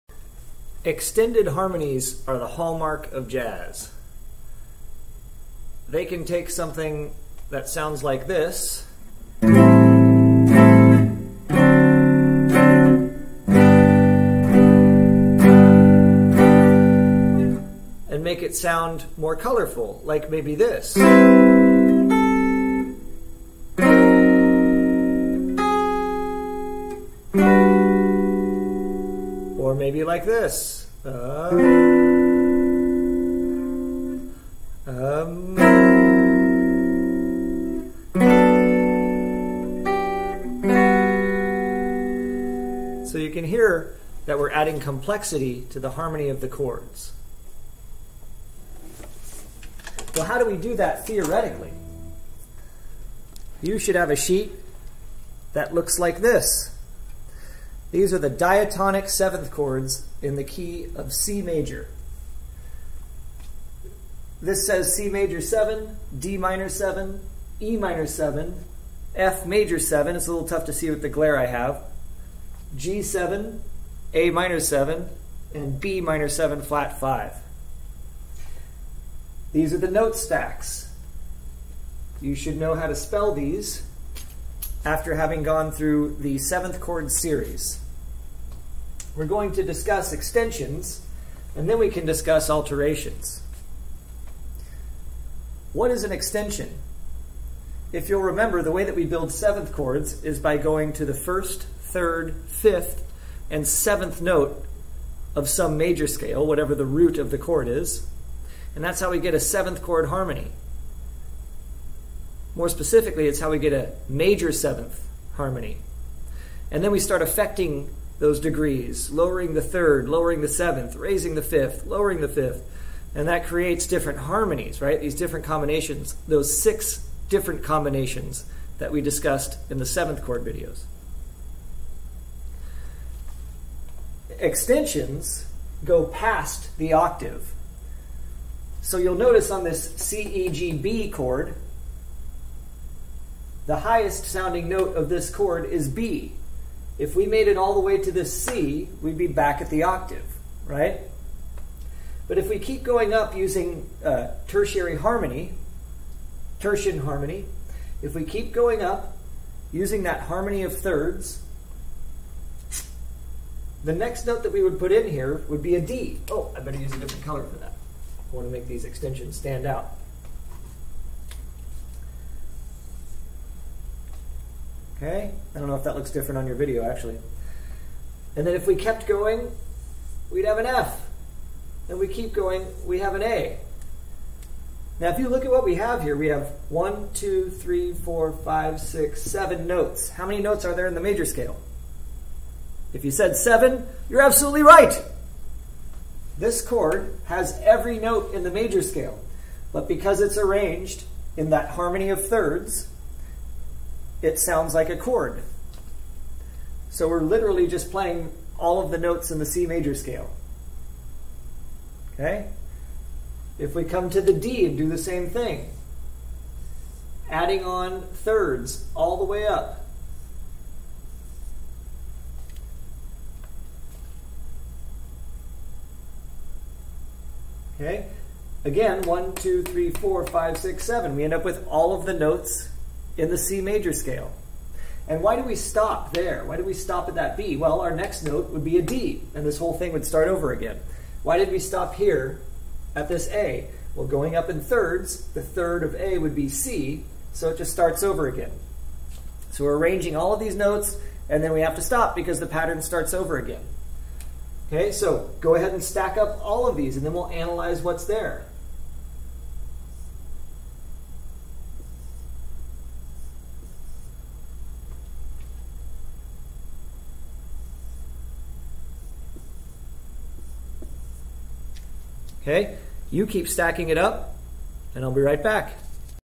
Extensions Vid1 Intro
This video is the first in the series on Extensions.  This video assumes that you have worked through the major scales, triads and 7th chord videos already and have a good understanding of those concepts.